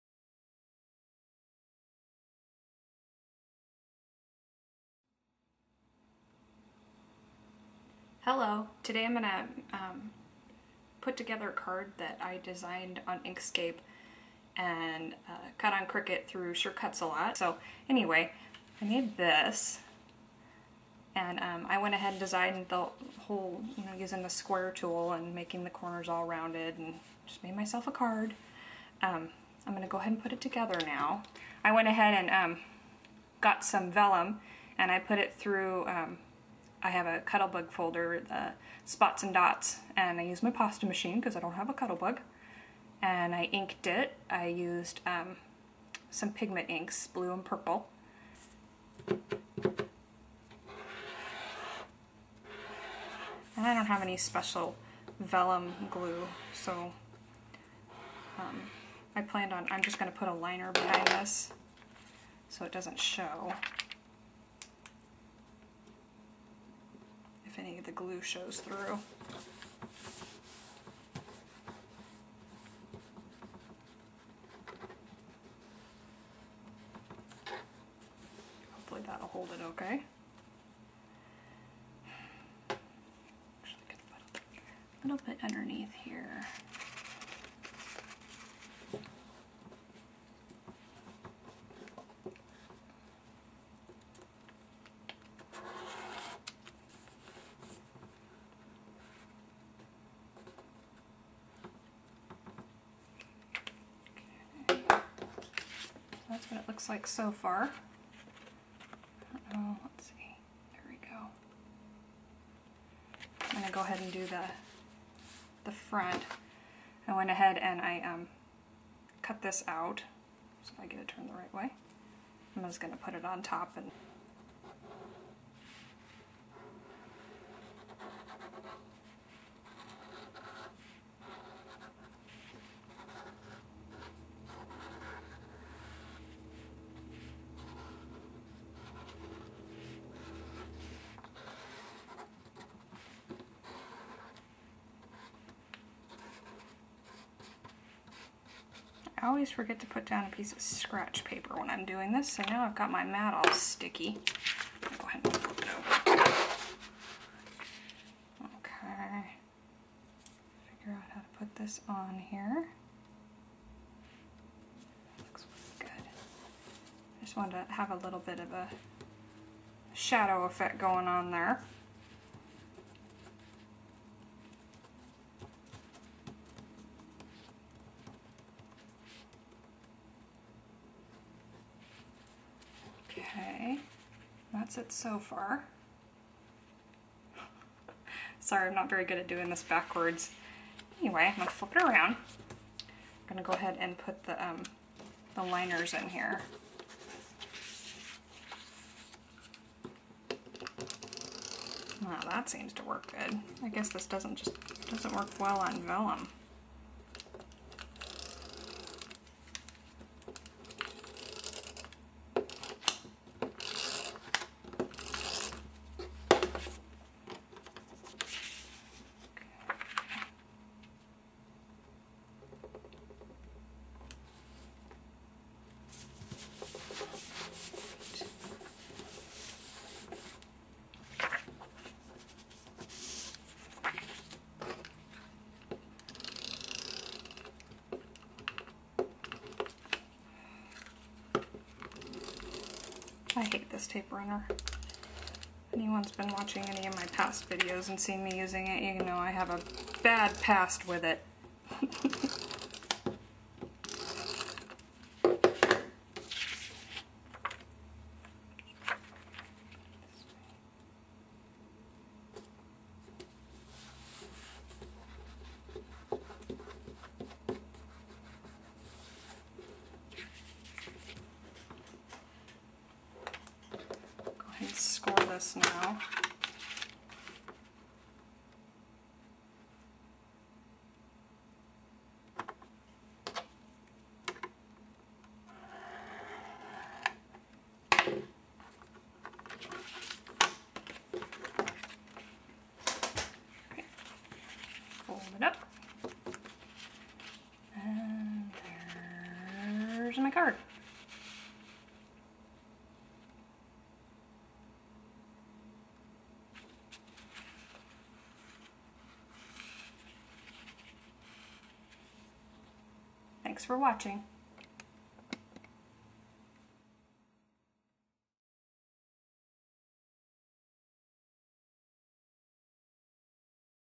Pretty flower card designed in Inkscape
Here's a card designed in Inkscape. Hope it might inspire you to get into inkscape and make some pretty cut files to cut in SCAL!